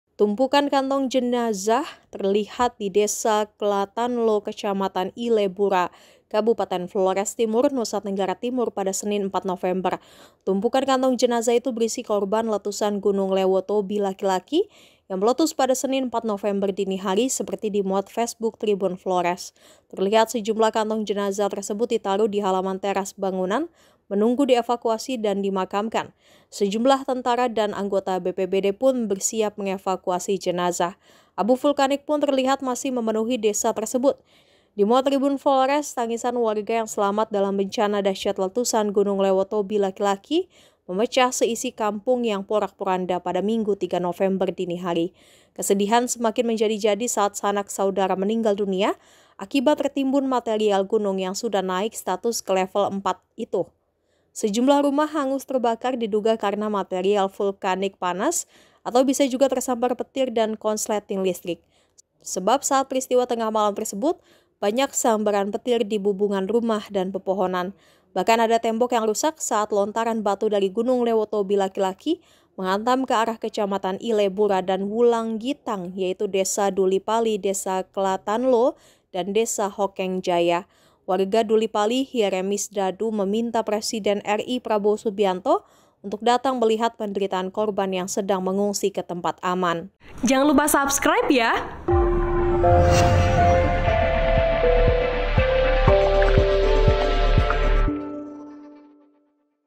Penampakan Tumpukan Kantong Jenazah Korban Letusan Gunung Lewotobi Laki laki
Tumpukan kantong jenazah menghiasi Desa Klatanlo, Kecamatan Ile Bura, Kabupaten Flores Timur, NTT, Senin 4 November 2024.

Tumpukan kantong jenazah itu berisi korban letusan Gunung Lewotobi Laki-laki yang meletus pada Senin (4/11/2024) dini hari seperti dimuat facebook TribunFlores.

Terlihat sejumlah kantong jenazah tersebut ditaruh di halaman teras bangunan menunggu di evakuasi dan dimakamkan.

Sejumlah tentara dan anggota BPBD pun bersiap mengevakuasi jenazah.

Abu vulkanik pun terlihat masih memenuhi desa tersebut.

Dimuat TribunFlores tangisan warga yang selamat dalam bencana dahsyat letusan Gunung Lewotobi Laki-laki memecah seisi kampung yang porak poranda, Minggu, 3 November 2024.

Kesedihan semakin menjadi-jadi saat sanak saudara meninggal dunia akibat tertimbun material gunung yang sudah naik status ke Level IV (Awas) itu.